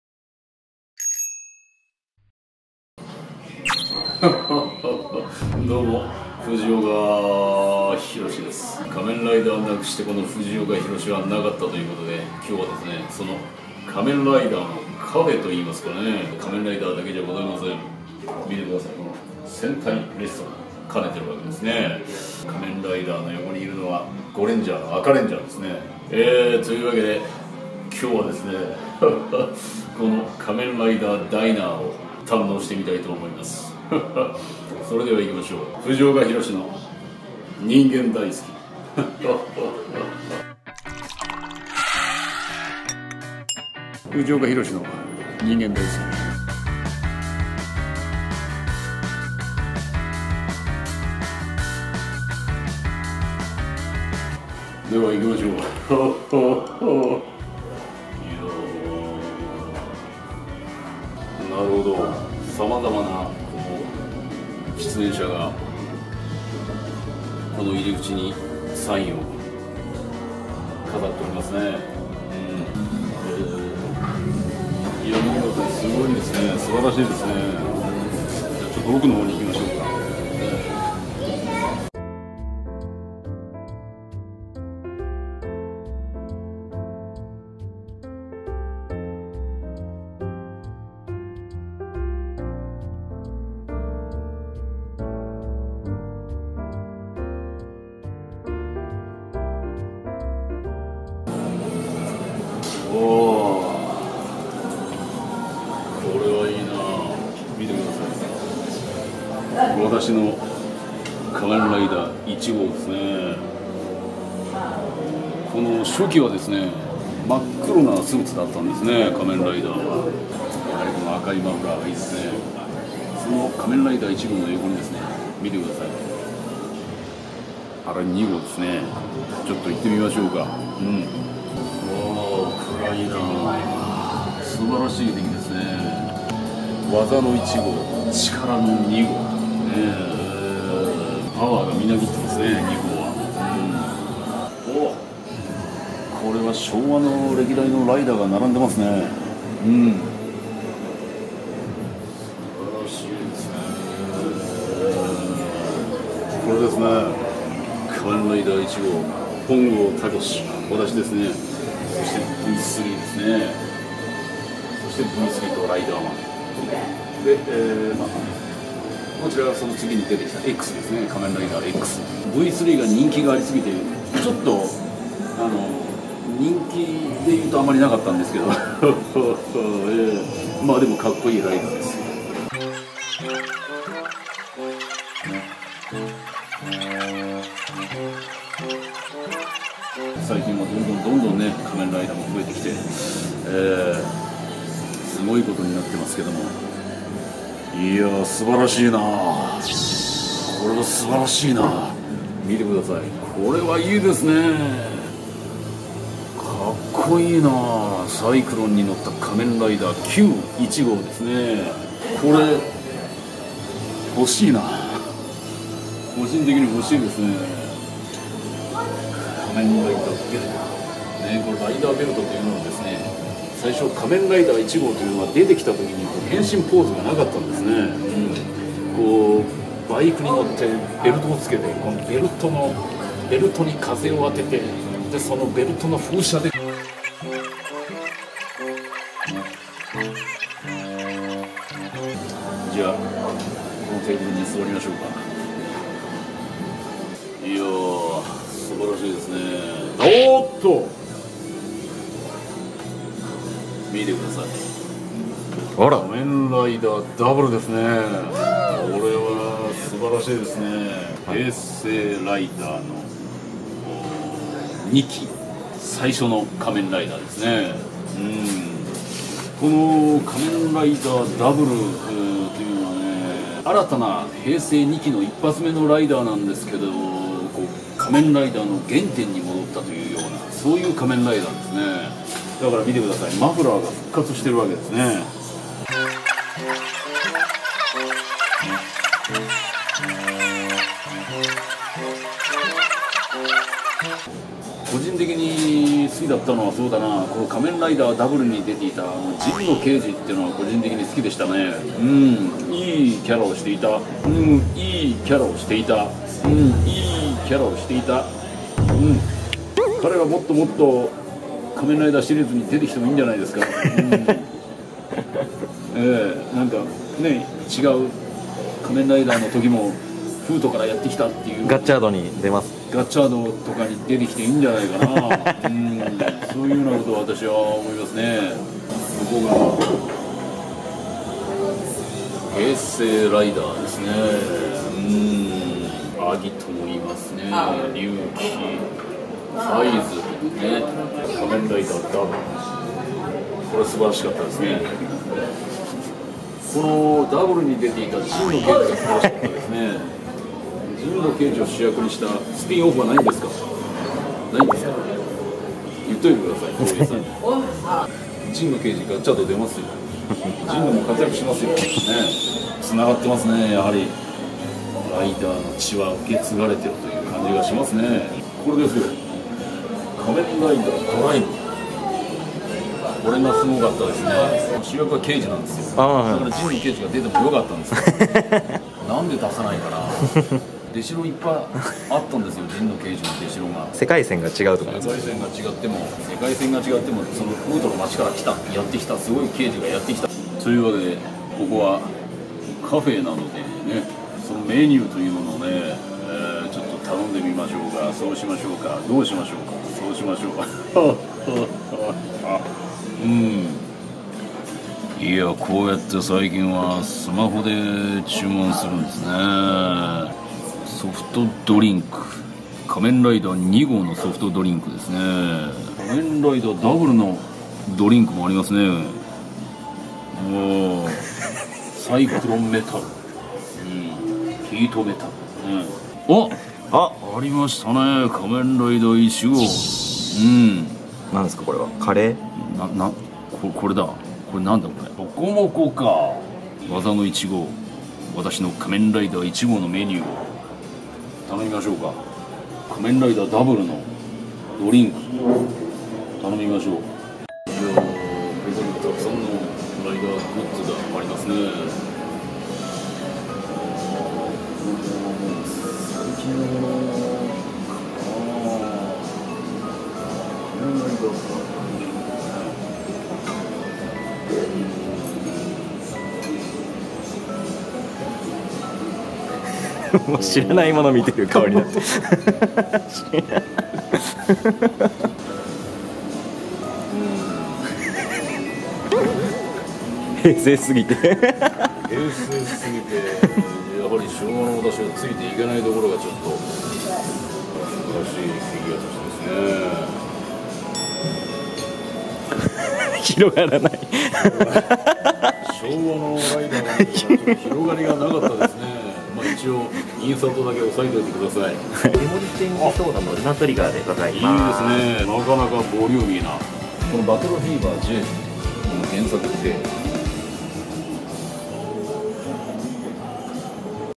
【藤岡弘、仮面ライダーカフェに行く】前編
【藤岡弘、の人間ダイスキ】シリーズ
池袋にある仮面ライダー・ザ・ダイナーに探検に行く藤岡弘、是非ご覧下さい